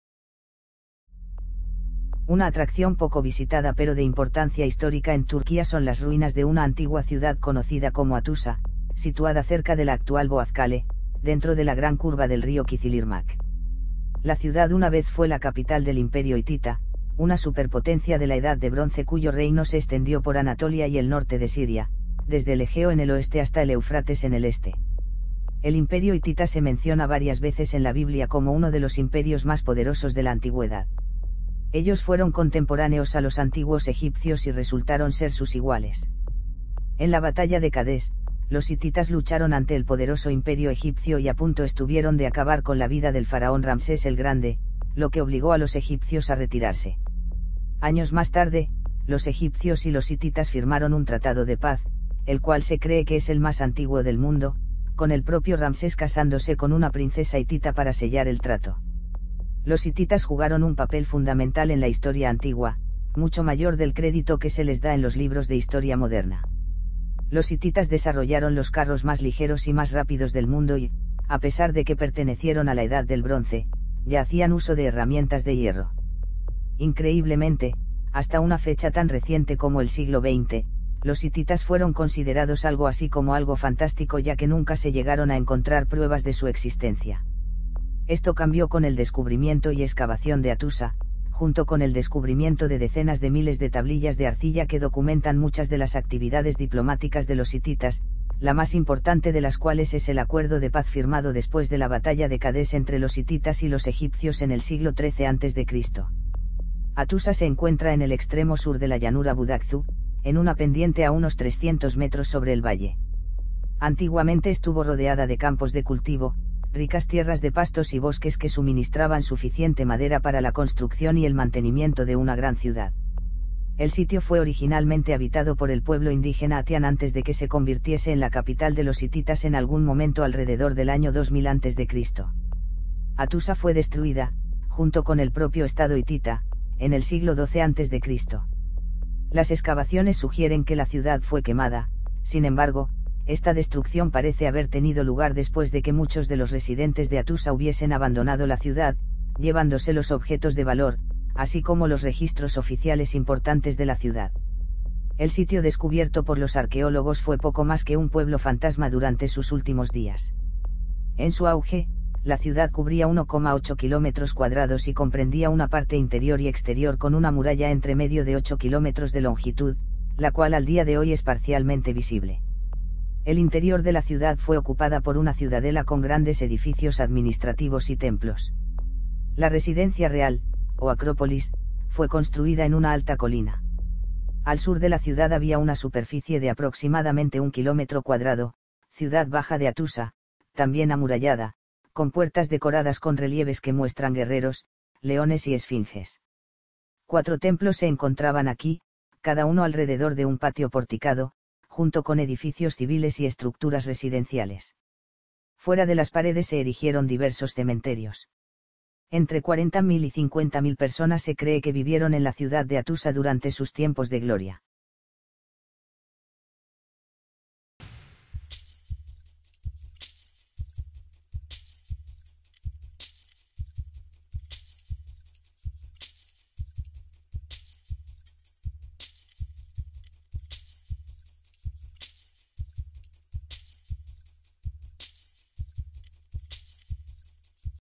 Hattusa, la antigua capital de los hititas | Turquia
Una atracción poco visitada pero de importancia histórica en Turquía son las ruinas de una antigua ciudad conocida como Hattusa, situada cerca de la actual Boğazkale, dentro de la gran curva del río Kizilirmak. La ciudad una vez fue la capital del imperio hitita, una superpotencia de la Edad de Bronce cuyo reino se extendió por Anatolia y el norte de Siria, desde el Egeo en el oeste hasta el Eufrates en el este.